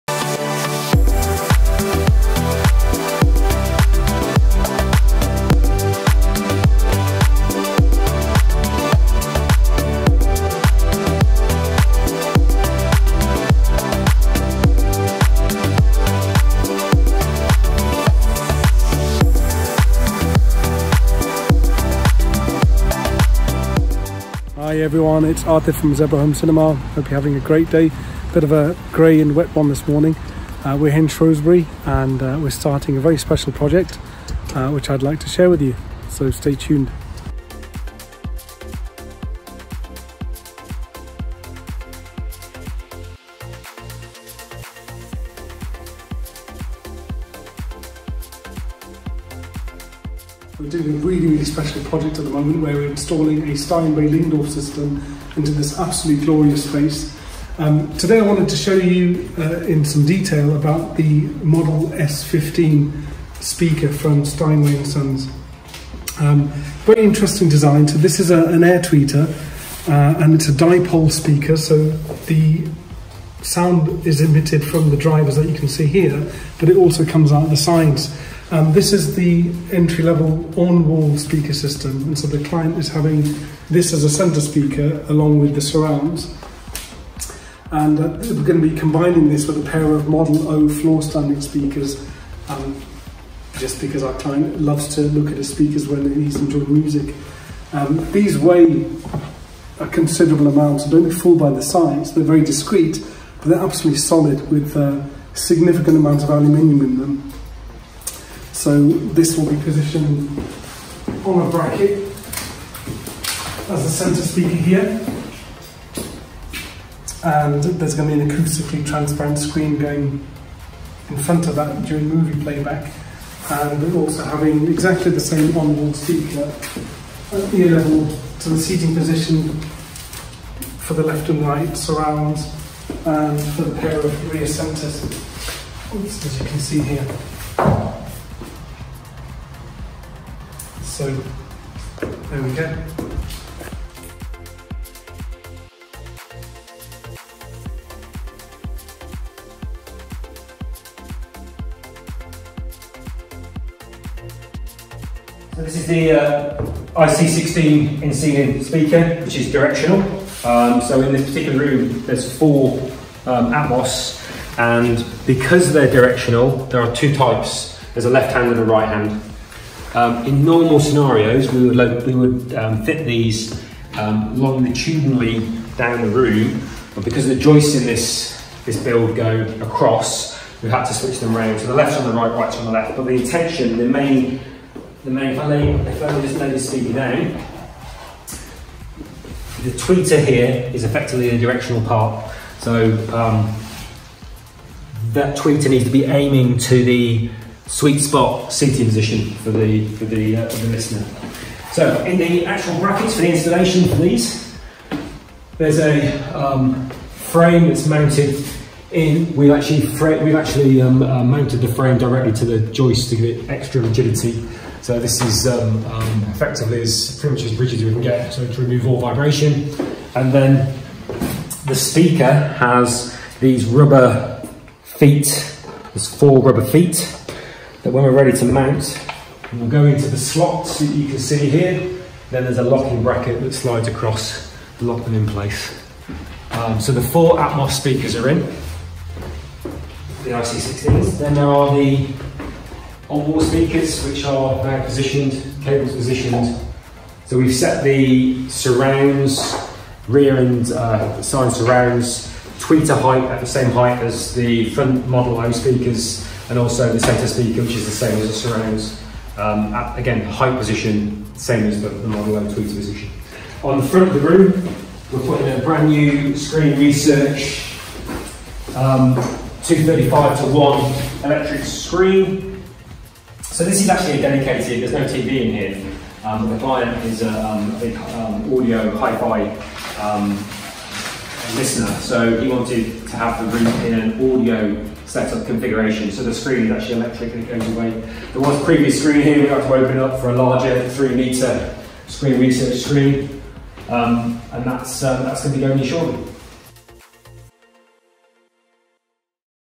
£120K STEINWAY & SONS HOME CINEMA - The Steinway Lodge - part 1 of 2.
Take an in-depth look into the installation process of the brilliant Steinway Lyngdorf audio systems in a glorious lodge space.

Our client recently completed a magnificent conversion of a 1930s power substation, having purchased the land and building from the power company. They created a beautiful Georgian entertainment lodge, the ground floor being a dedicated music and home cinema room.

Check out the full case study by heading over to our